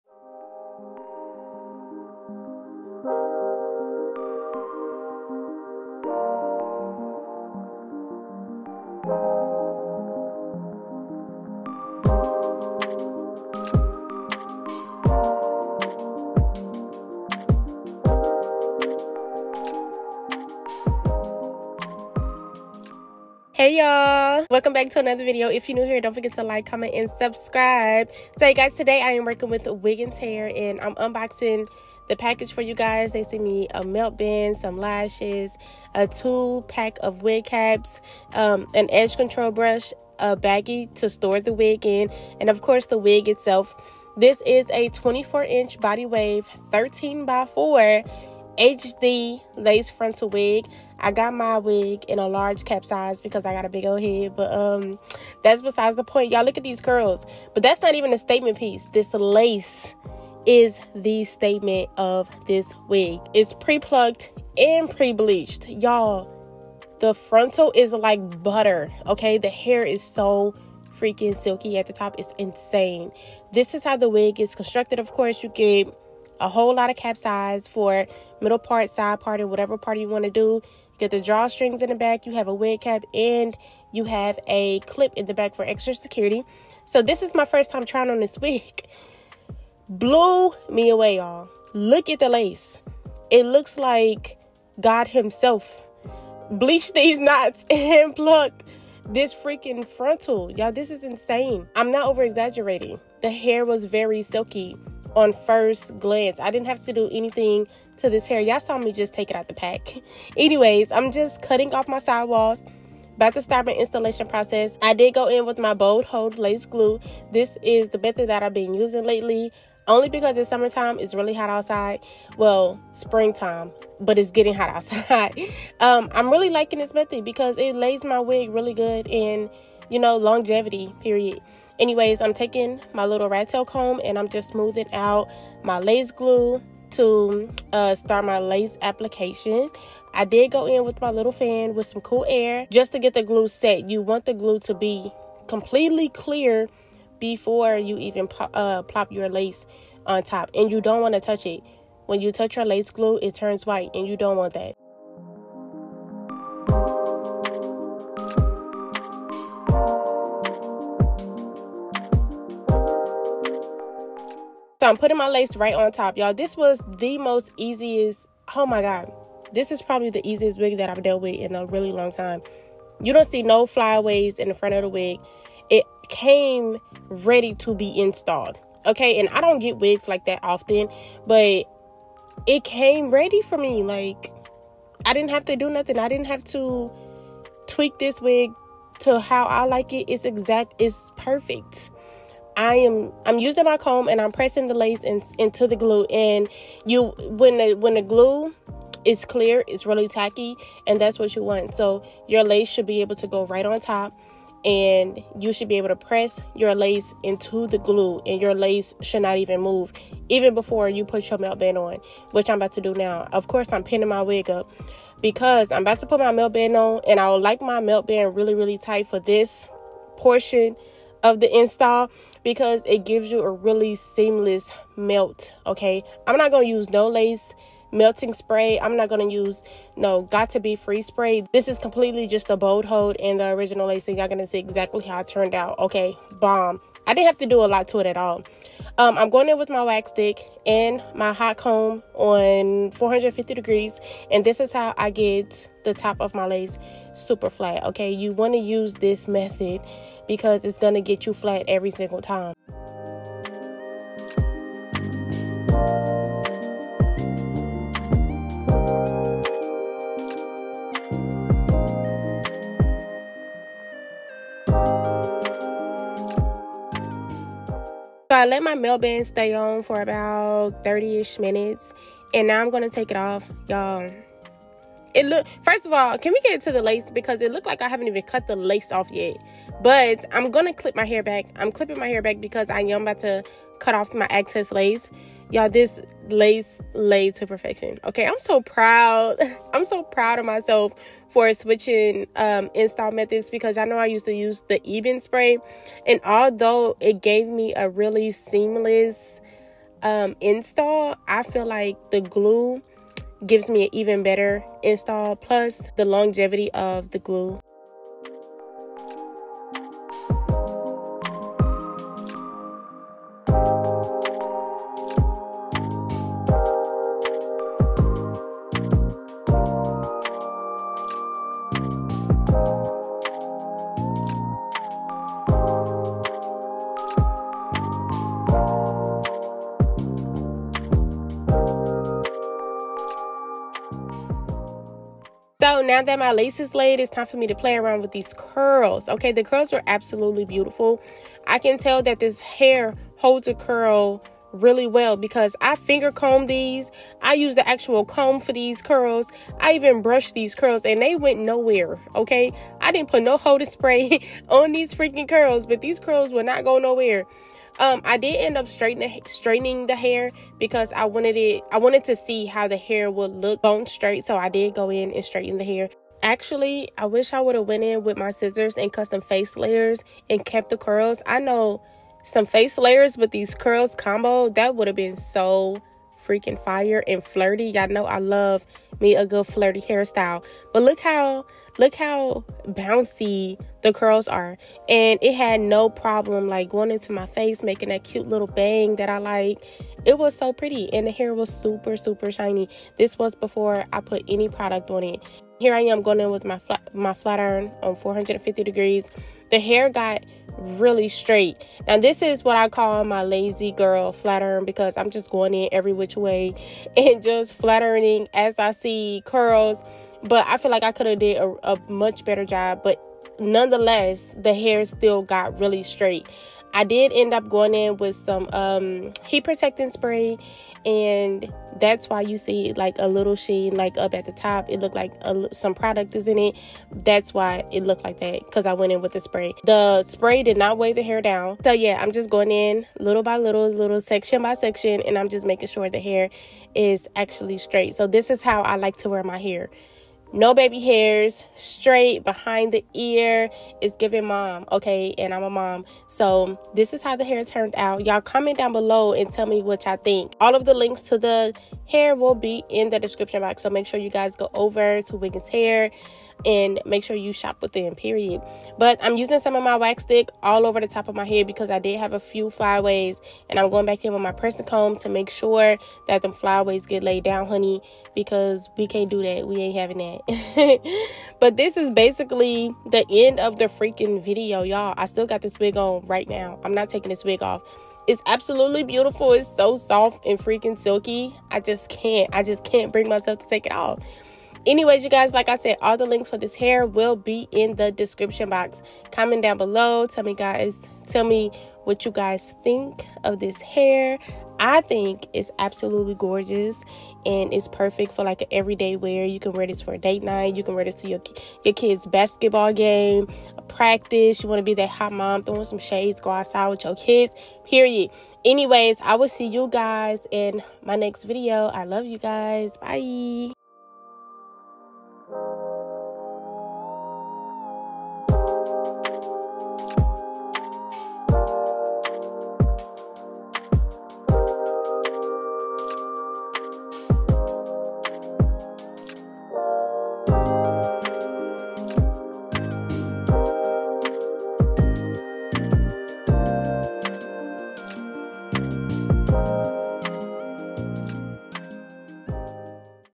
This Wig EATS! Quick and Easy Install and Style!! ft Wiggins Hair
▬▬▬▬▬▬▬▬▬▬♛Wiggins Hair♛▬▬▬▬▬▬▬▬▬▬▬
Hair details: 24inch body wave 13x4 HD lace frontal wig

3.NEW V part wig! More Natural➤